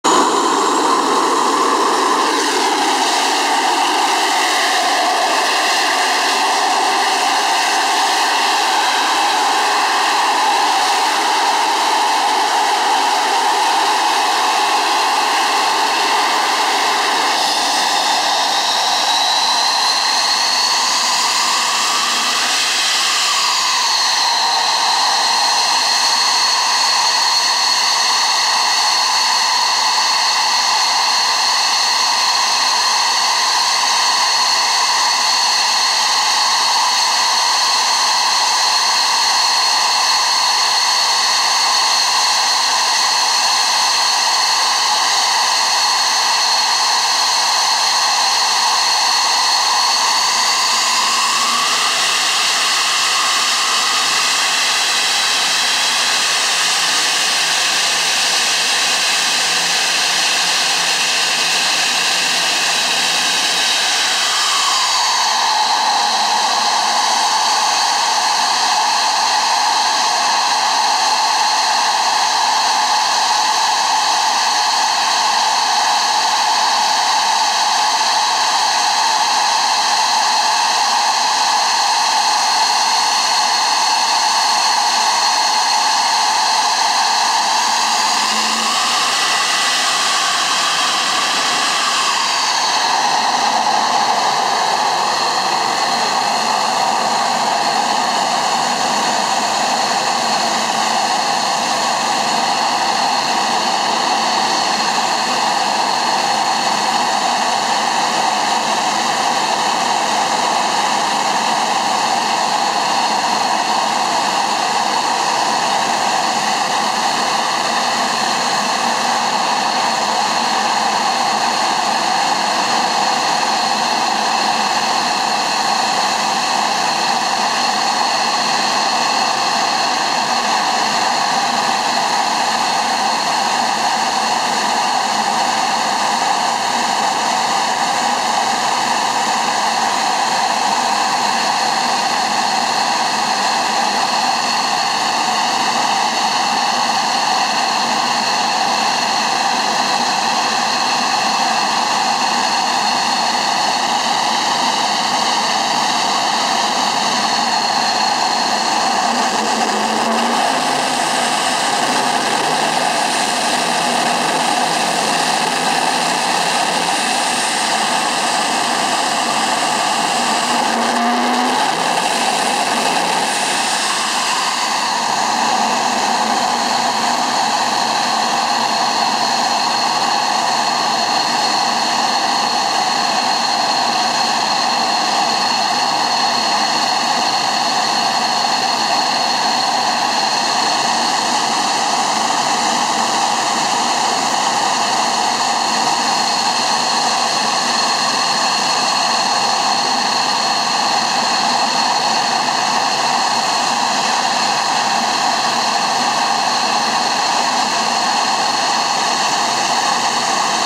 Mirage 2000D Démarrage et verifs avion avant decollage
Demarage M53 et verifs avion avant decollage de Valenciennes
Ce 14 juillet 2013